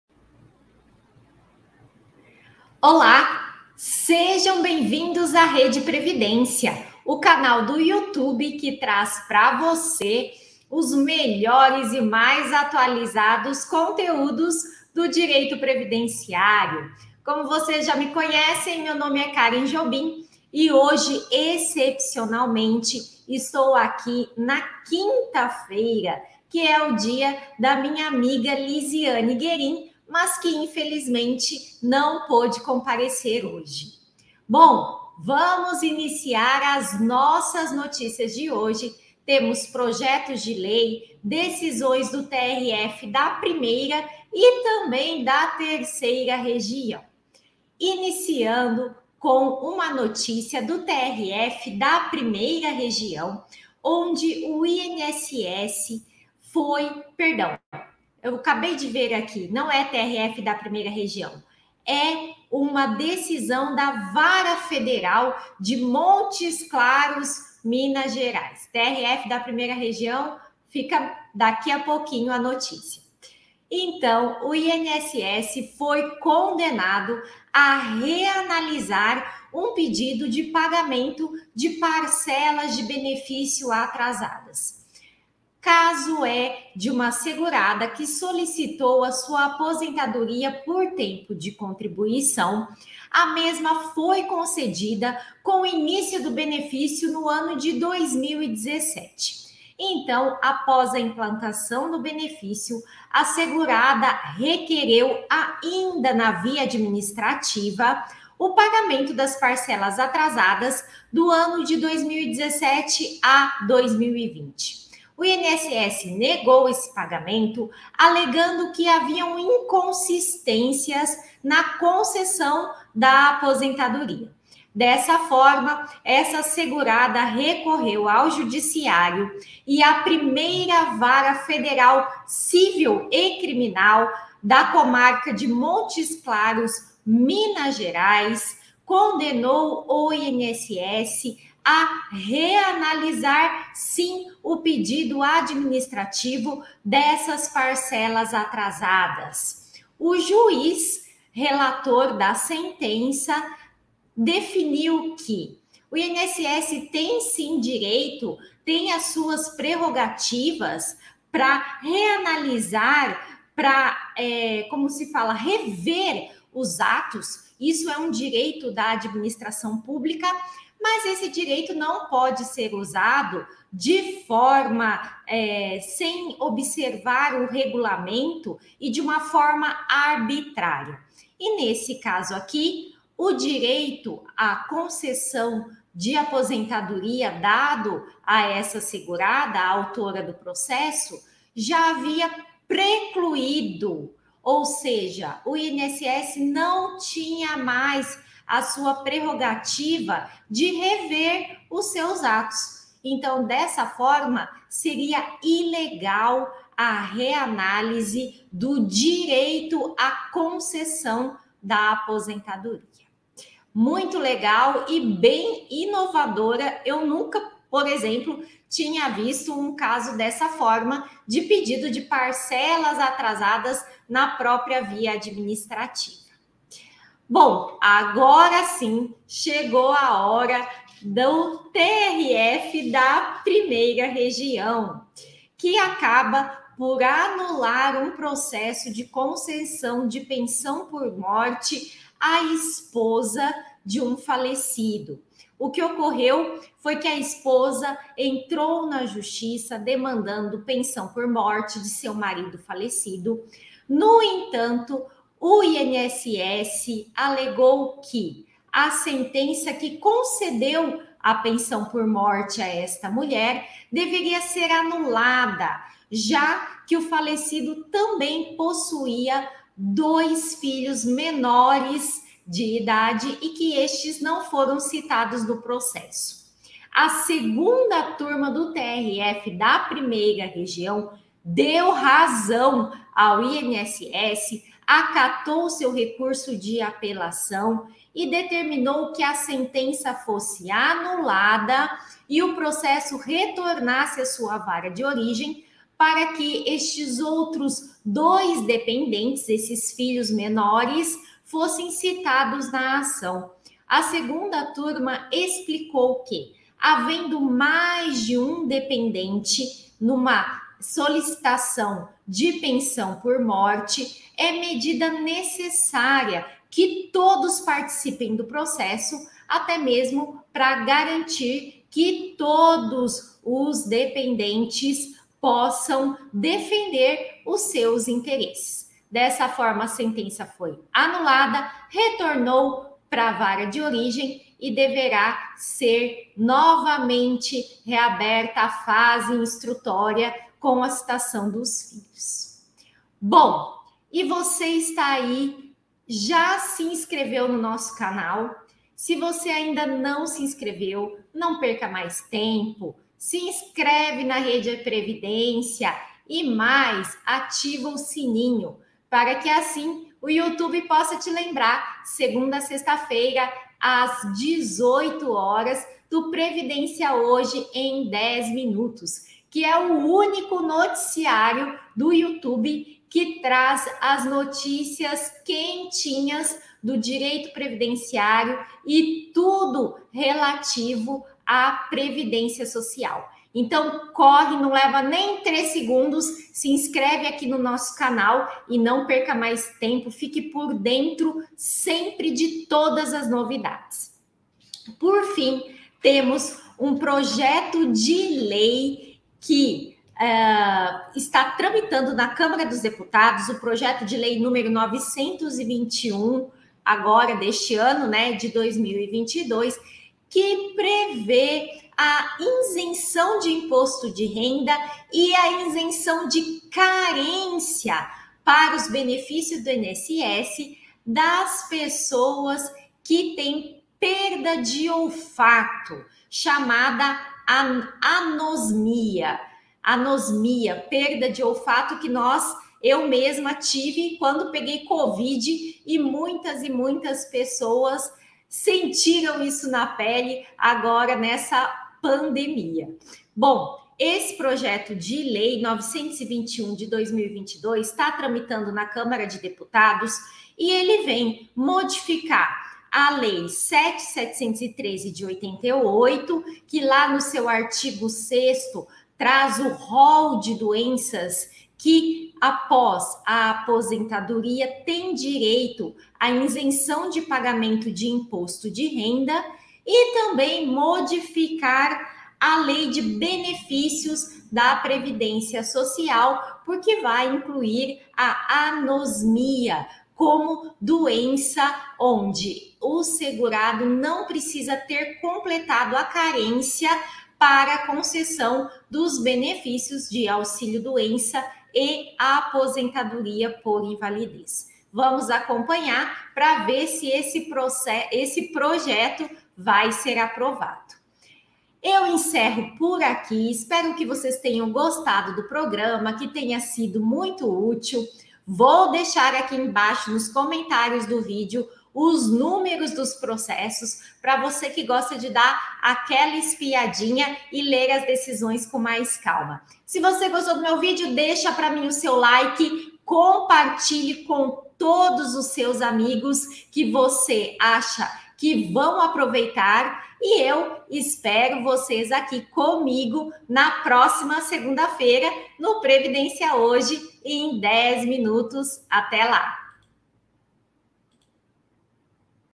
Voc6e viu? Projeto prevê isenção de carência do INSS para pessoas com perda do olfato e mais....
- Projeto prevê isenção do imposto de renda e carência do INSS para pessoas com perda do olfato;

- INSS deve reanalisar pedido de parcelas atrasadas de uma aposentadoria;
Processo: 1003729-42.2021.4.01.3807

- TRF1 anula concessão de Pensão por Morte devido a não citação dos filhos no processo;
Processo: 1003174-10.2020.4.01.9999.